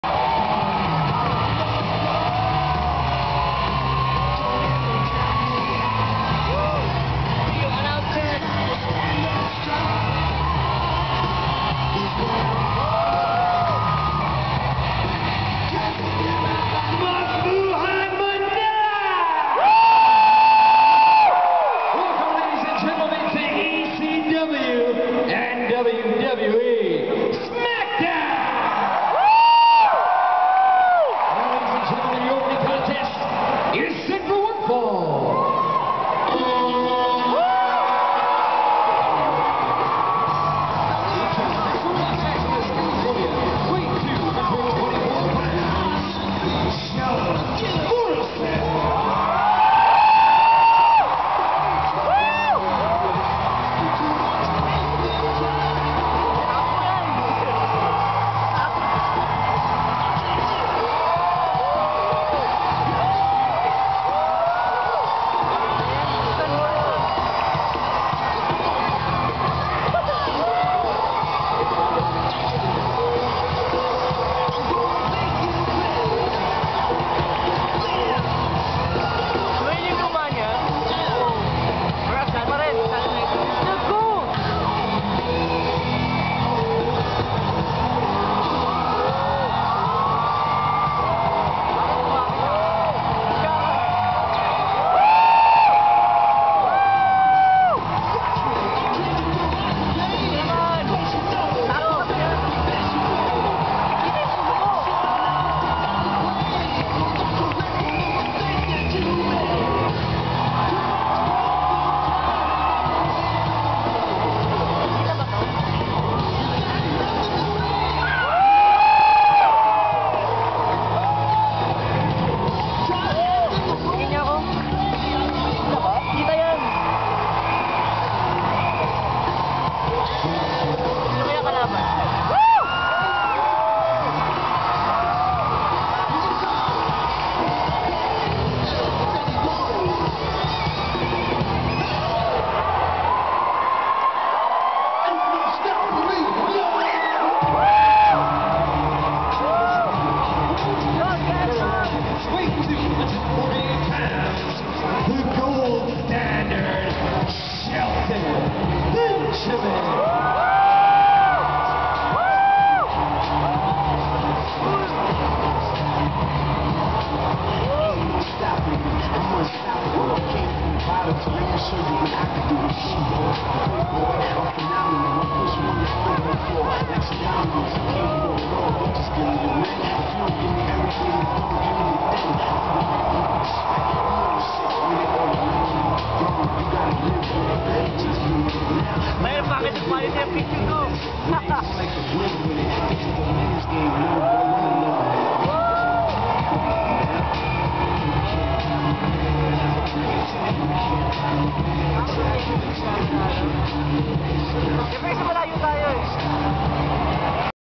John Morrison vs. Shelton Benjamin Live Event
The entrances of Morrison and Benjamin in WWE's live event at the Philippines. Sorry about the shaky camera, it was my first time in a live event. This match is the first match of the night.